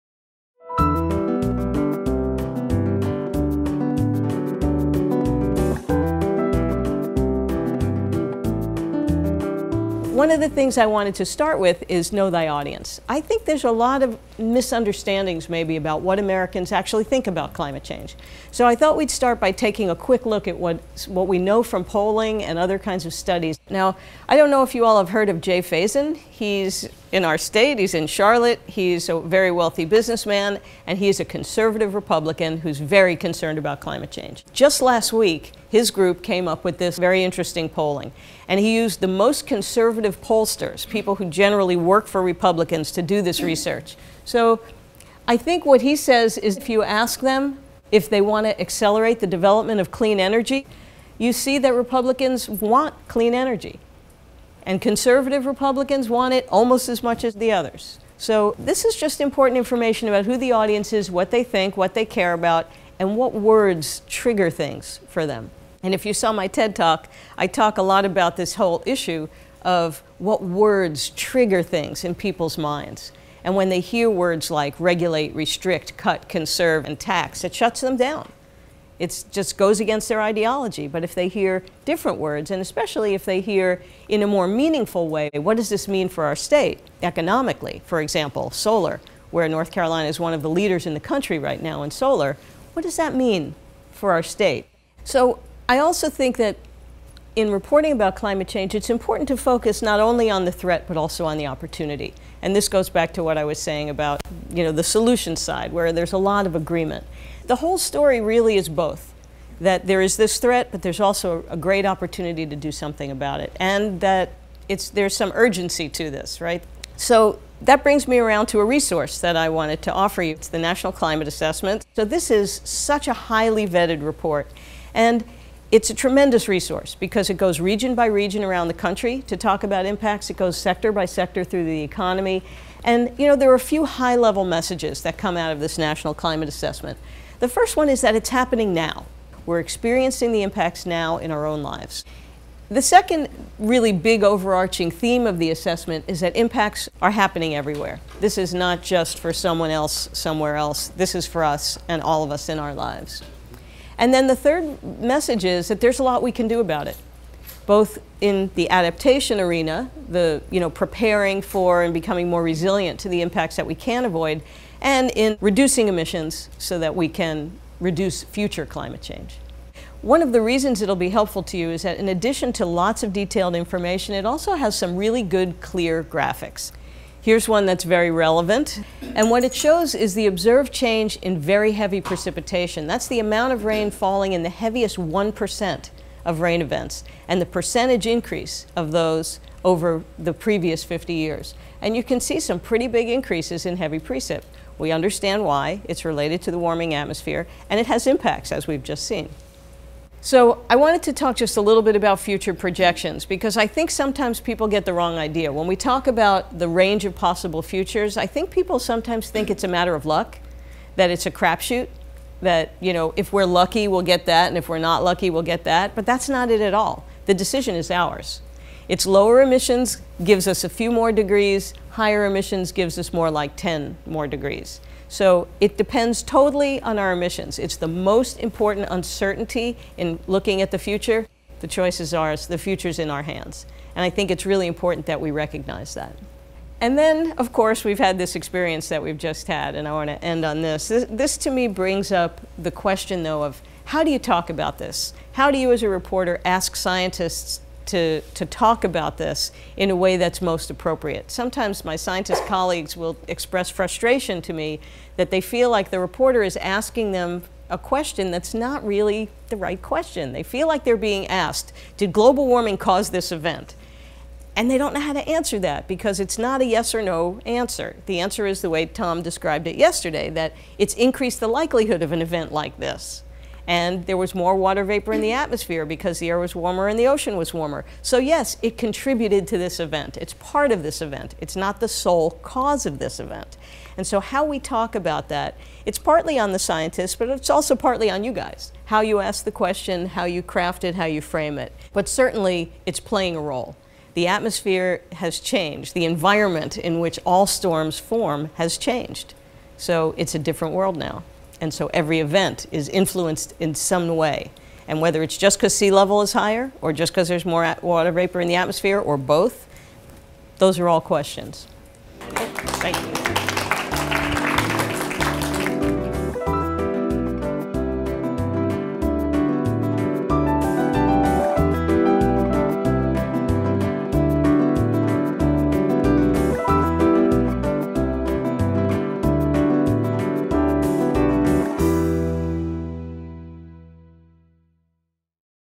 Susan Hassol, Climate Change Workshop for Journalists: Communication
This video shows portions of a workshop on Coastal Climate Change for Media Professionals that was held in October 2015 in Beaufort, N.C.  The event was organized by the North Carolina Coastal Federation, Climate Communication, the Southeast Climate Science Center, and Climate Central to provide a forum for media professionals to learn about climate change and its impacts in coastal North Carolina.